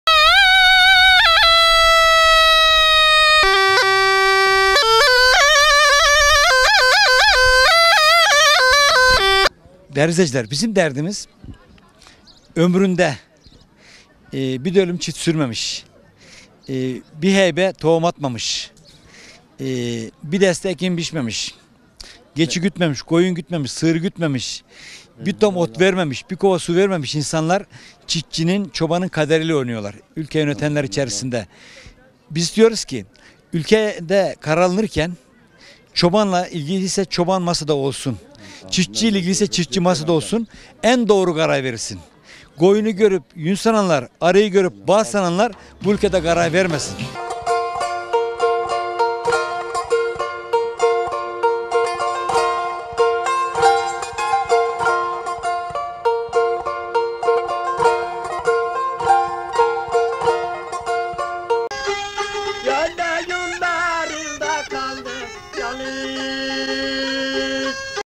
ÇİFTÇİNİN ÇOBANIN DERDİ
ÇOBANIN DERDİNİ ÇÖZÜN, ÜRETİMDEN ÇEKİLMESİN. AÇ KALIRSINIZ.